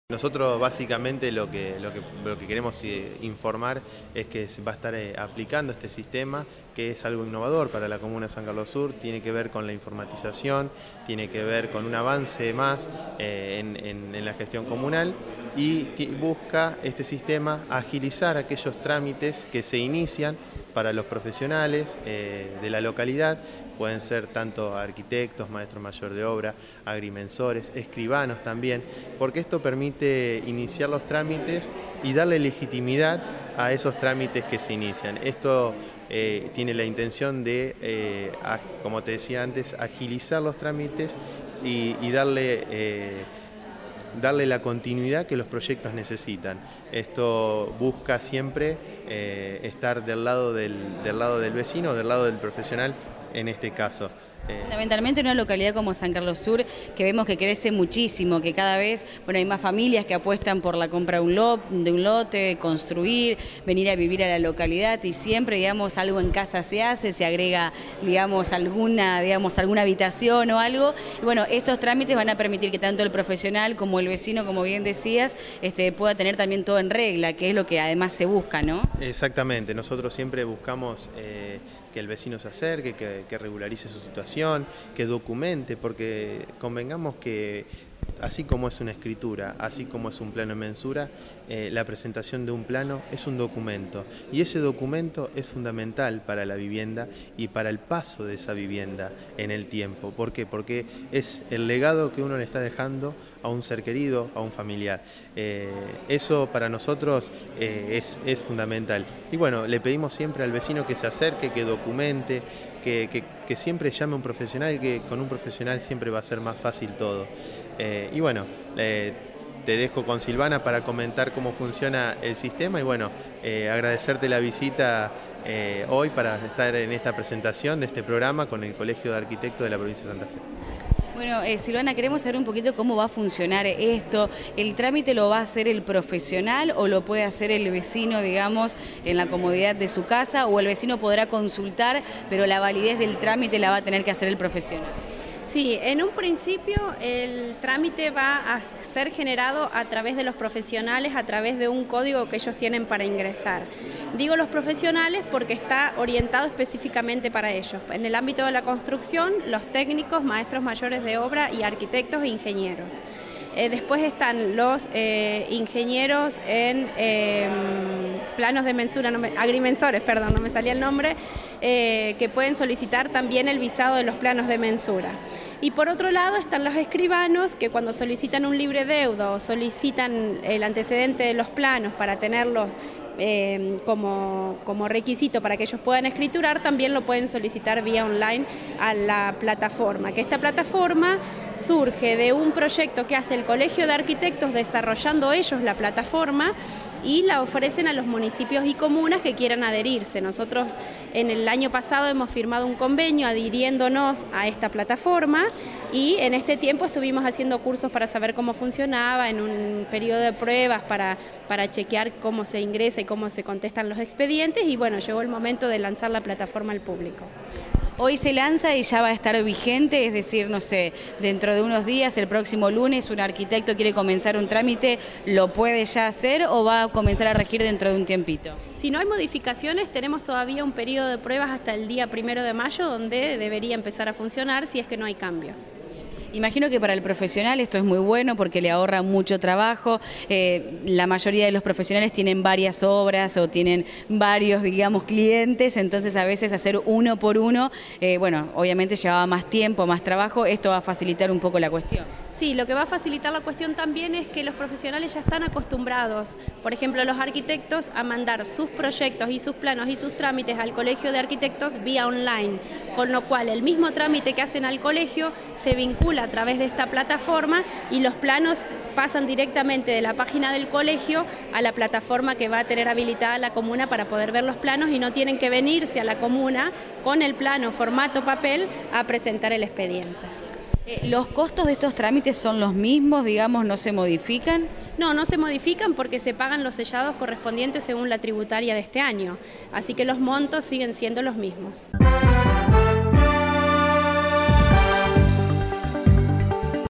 Arq Santiago Walker y Silvana Gullino
La comuna de San Carlos sud presentó el sistema GESTO